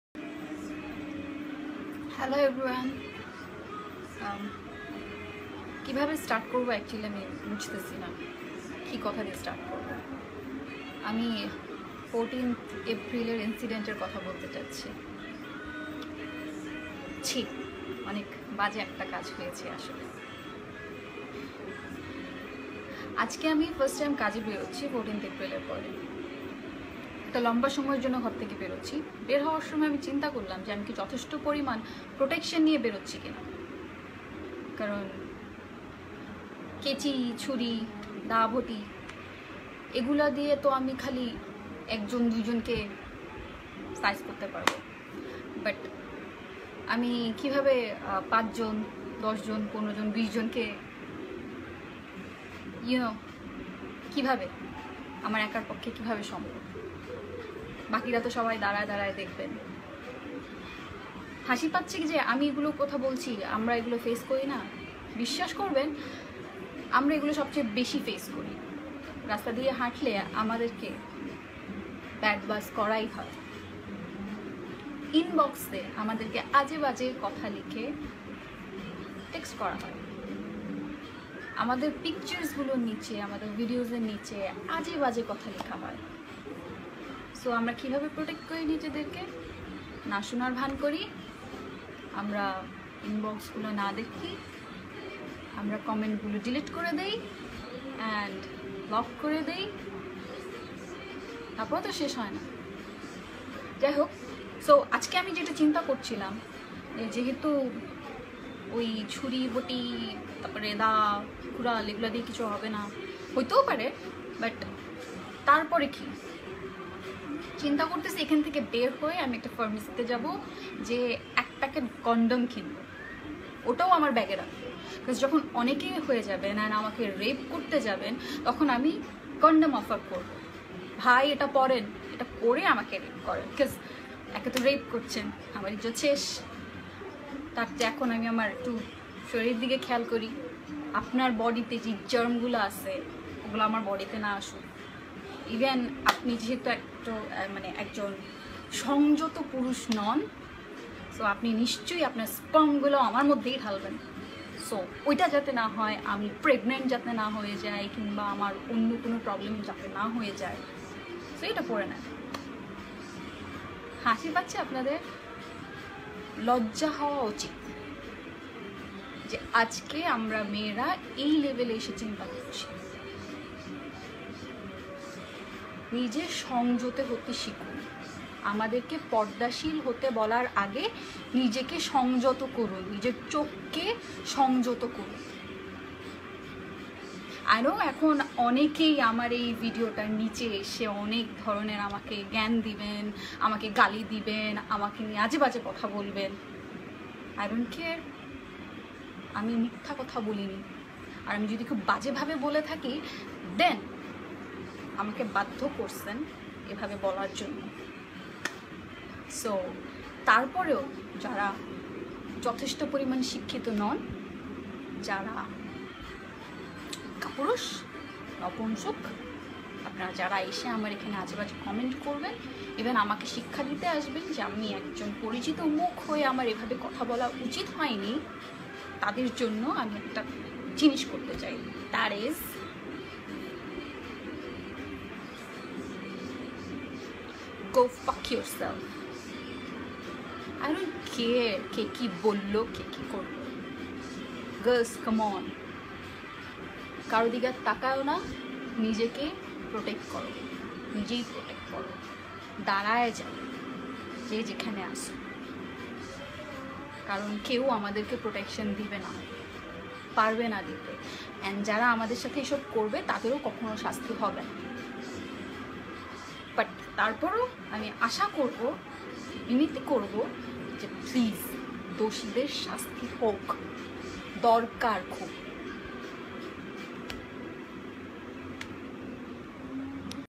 ধর্ষকদের জন্য ব্যাগে কনডম রাখার আহ্বান টয়ার | JagoNews24.com
নারীদের প্রতি সবসময় ব্যাগে কনডম রাখার আহ্বান জানিয়েছেন মডেল ও অভিনেত্রী মুমতাহিনা টয়া। এ বিষয়ে নিজের ফেসবুক অ্যাকাউন্টে তিনি একটি ভিডিও পোস্ট করেছেন।

ভিডিওতে টয়া বলেন, সে সহ মিডিয়াঙ্গনে কাজ করা নারীদেরও রাস্তা ঘাটে এমনকি সামাজিক যোগাযোগ মাধ্যমগুলোতে এমন পরিস্থিতির শিকার হতে হয়। রাস্তায় বখাটেদের কথাগুলো না শোনার ভান করে কিংবা সামাজিক যোগাযোগ মাধ্যমে তাদের কমেন্ট মুছে দিয়ে কিংবা ব্লক করে এই পরিস্থিতি এড়াতে হয়।